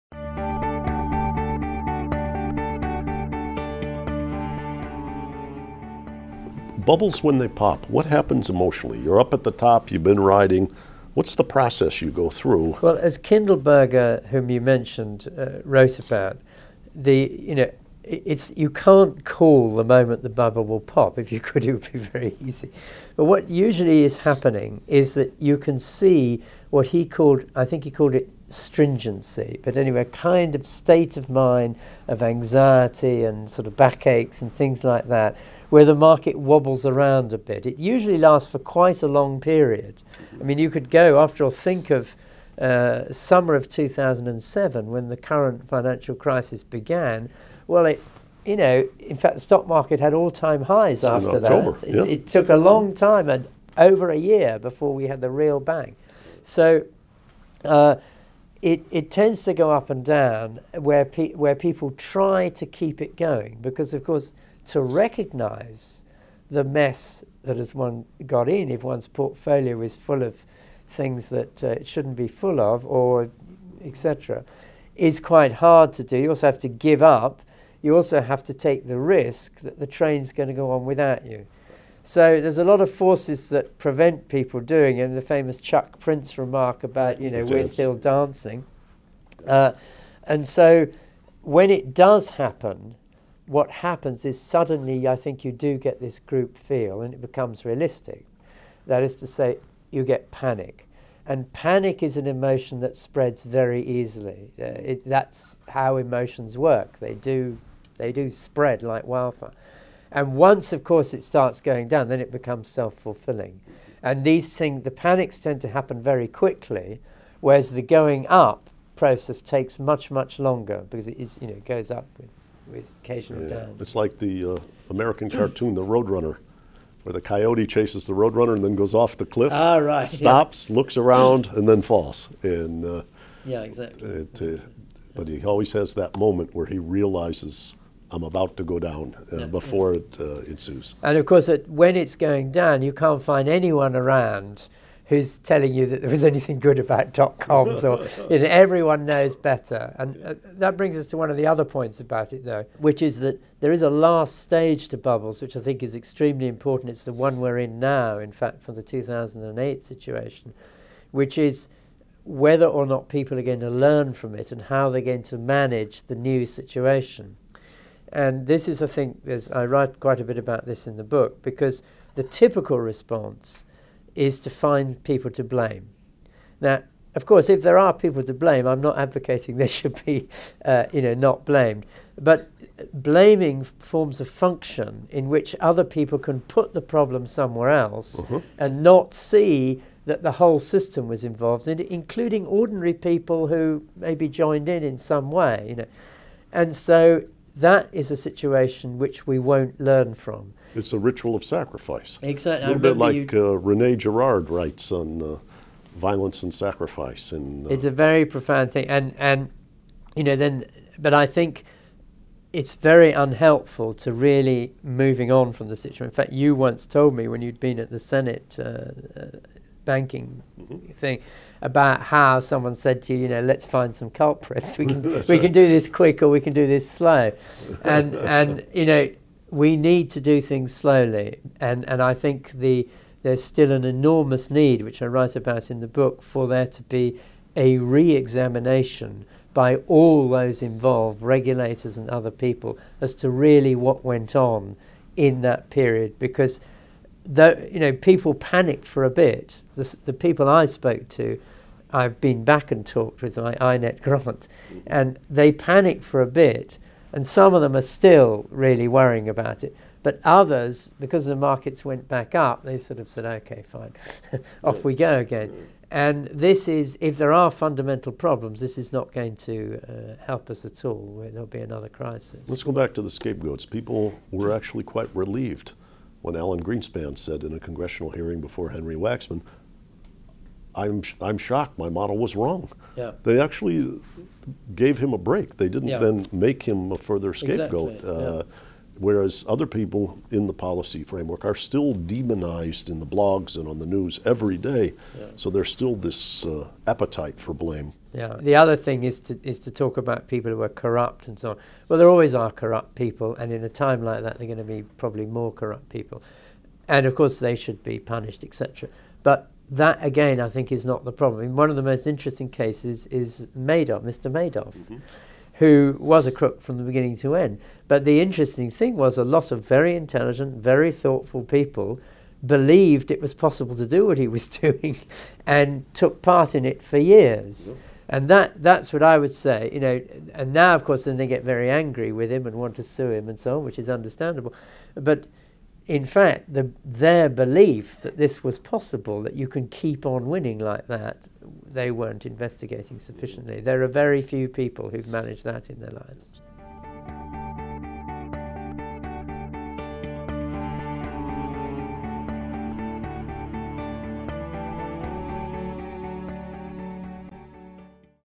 David Tuckett - The Way Down: When Bubbles Pop
David Tuckett explores how stories affect when the market changes after bubbles pop. He notes that markets often continue to go up after crises begin, supporting the notion that it's stories, and not simple statistics, that drive markets.

This is part 4 of a 6-part INET interview.